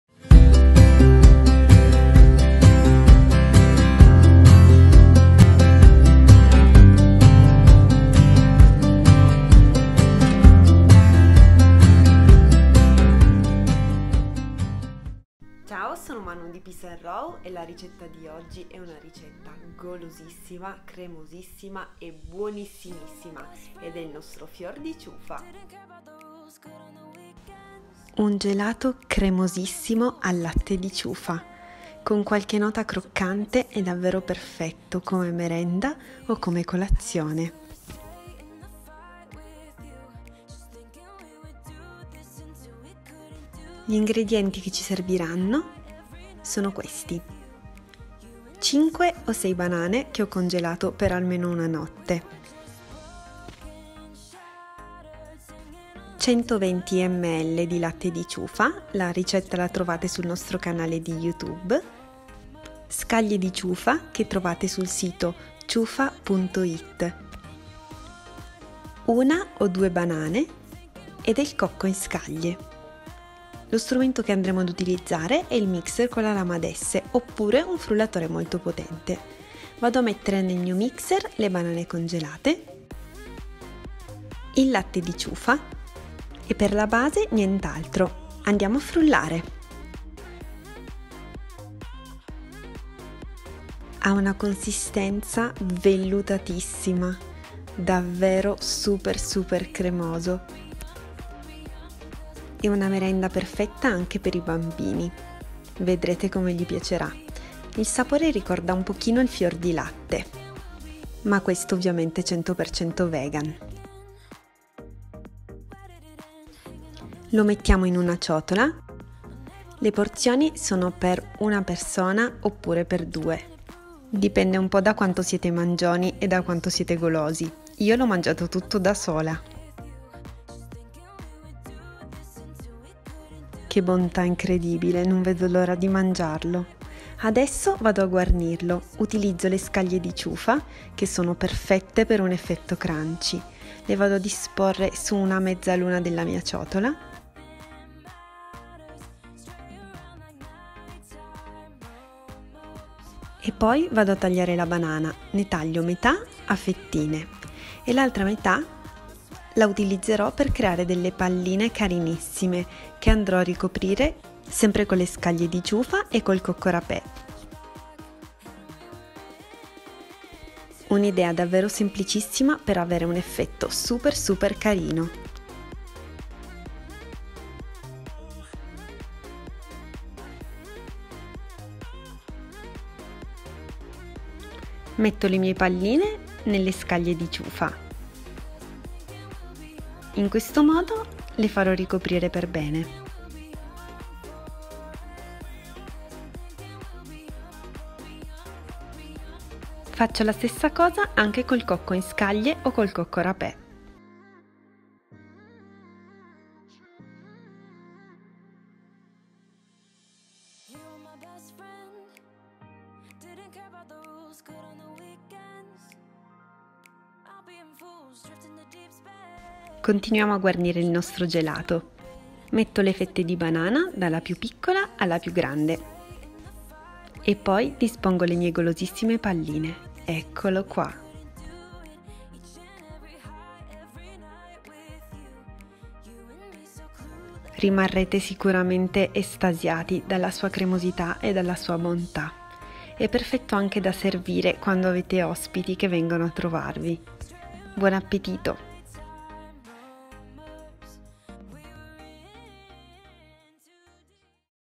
Fior di Chufa Raw Nice Cream : come fare un gelato 100% vegetale vegano e crudista
Un gelato cremosissimo al latte di Chufa con un effetto super crunchy!